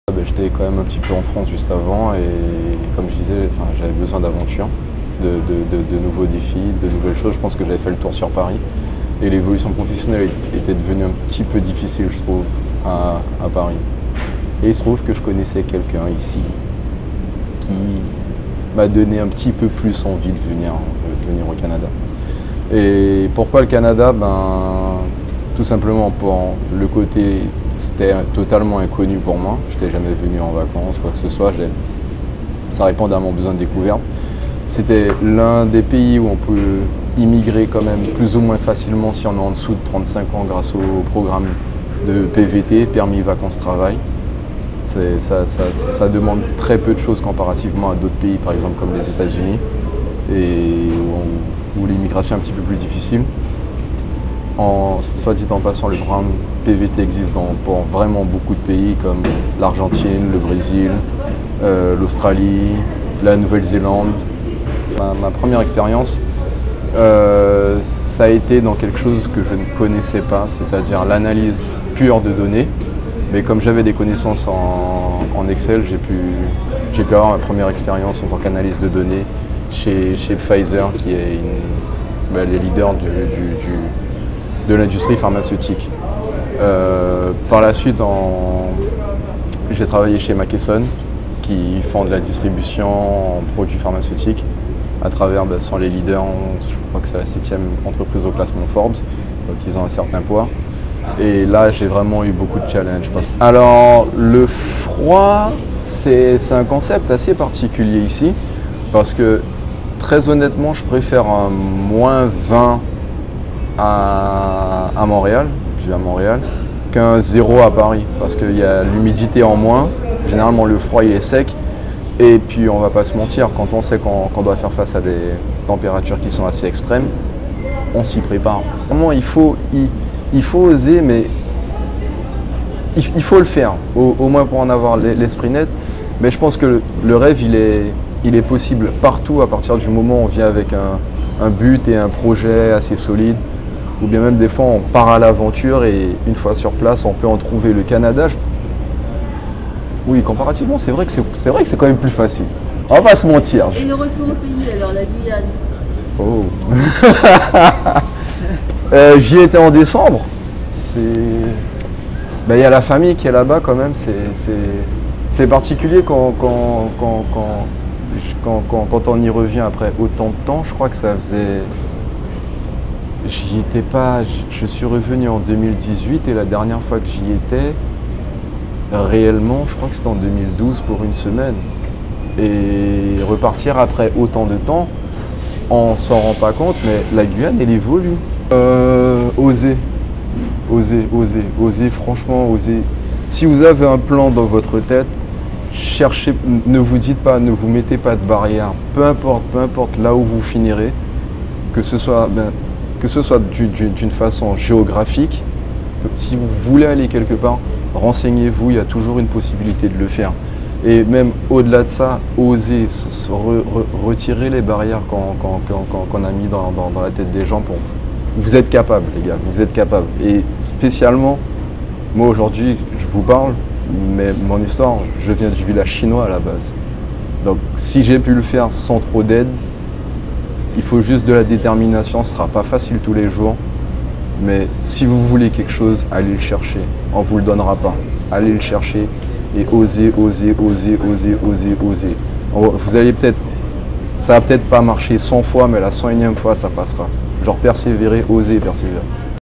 Le rêve Canadien de Pascal Bafau Bruné
Pascal Bafau Bruné vit et travail à Montréal au Canada. Un rêve qui est devenu réalité pour le jeune Guyanais de 32 ans, qui a passé toute son enfance au Village Chinois à Cayenne avant d'entreprendre des études universitaires pour s'en sortir. Un bel exemple pour la jeunesse.